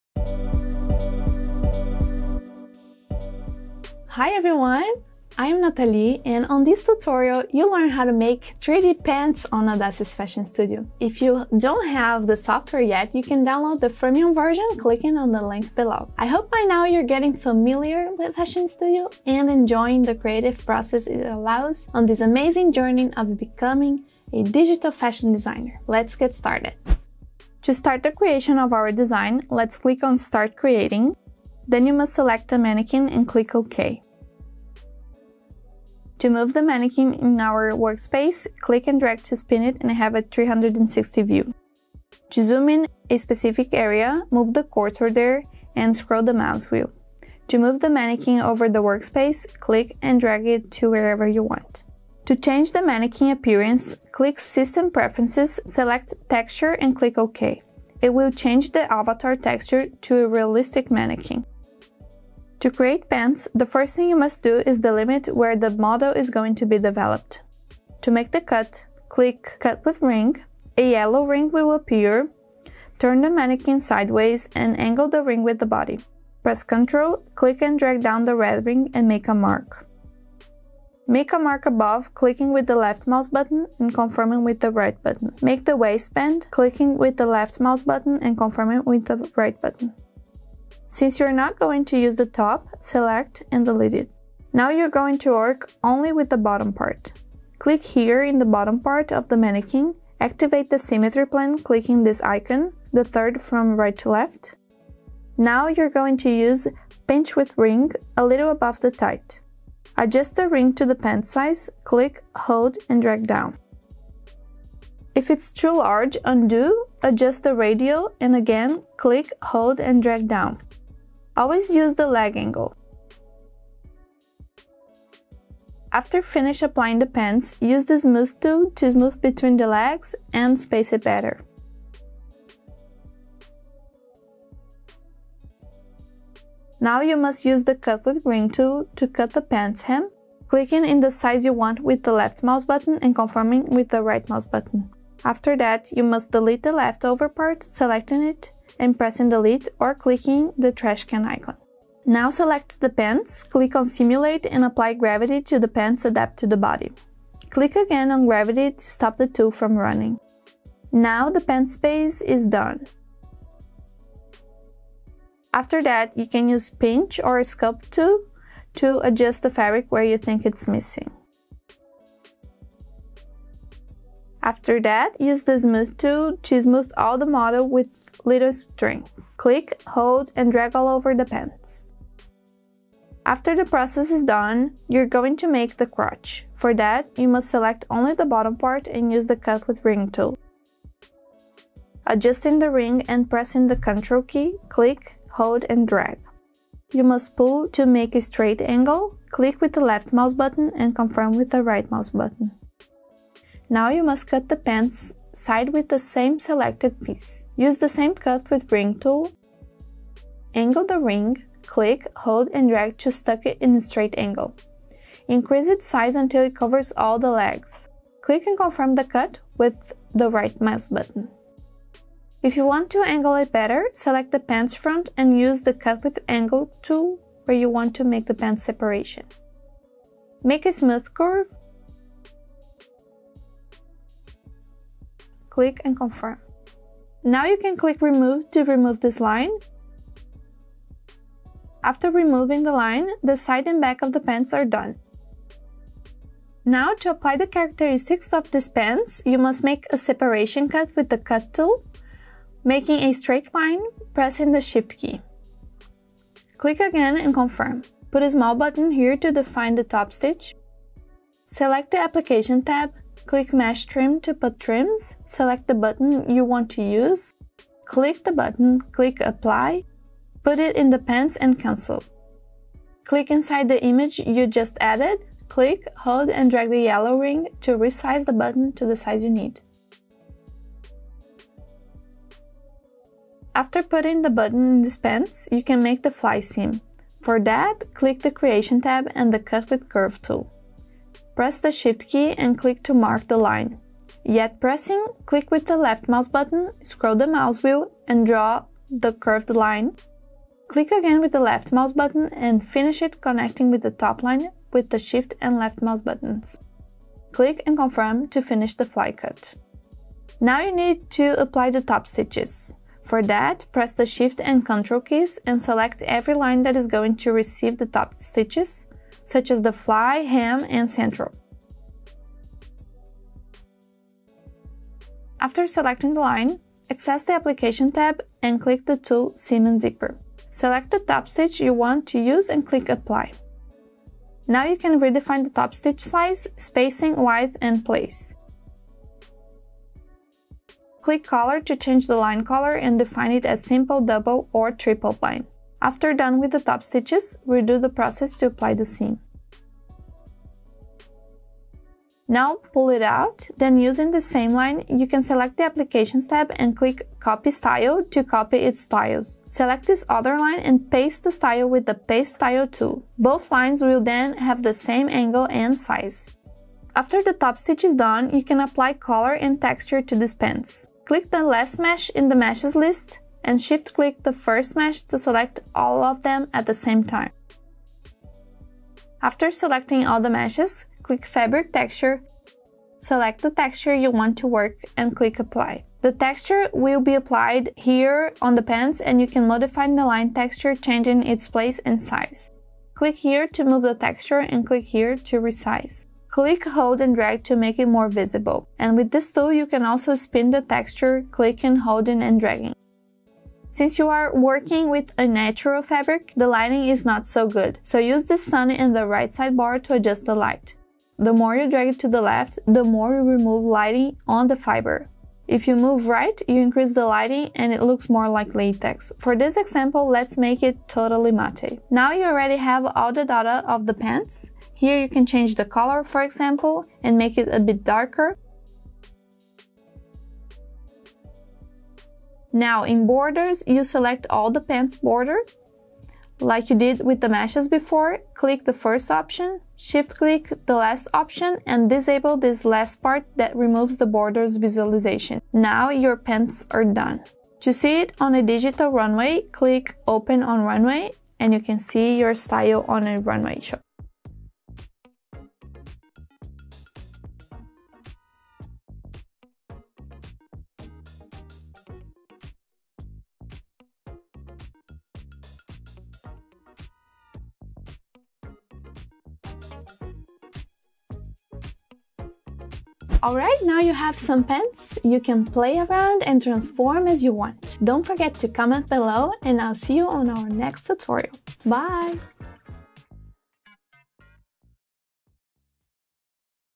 How to create 3D pants on Audaces Fashion Studio | Digital Fashion Tutorial #2
On Audaces Fashion Studio, creating 3D outfits is so easy that everyone can be a fashion designer!